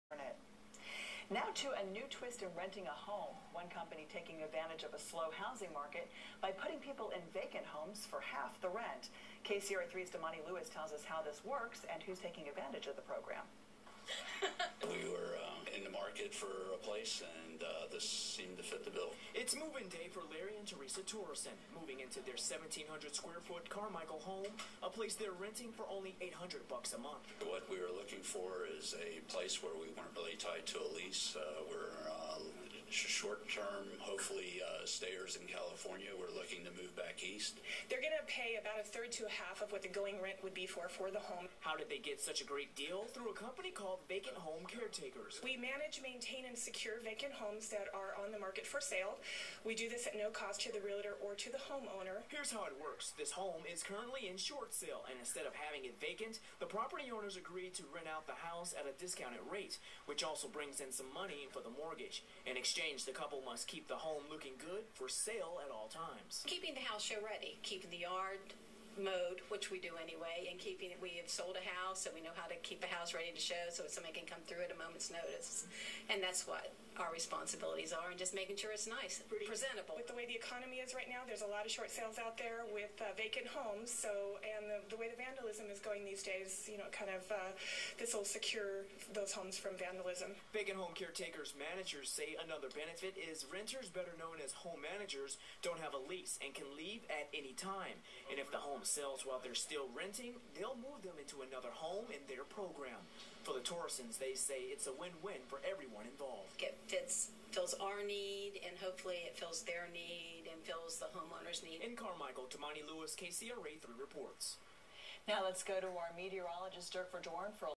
Vacant Home Caretakers on KCRA 3
I am a Realtor, and one of my concerns with trying to market a vacant home is the potential for vandalism and the likelihood that the home and yard will not be adequately maintained.  I learned of Vacant Home Caretakers (VHC) and got to know Jodi Guevara, and I have found it to be the perfect solution.  VHC will place a Home Manager in the home, who will stage it with their own preapproved furnishings, will keep it sh ready inside and out, and will pay the utilities.  All at no cost to the seller or the listing agent.  I love this service!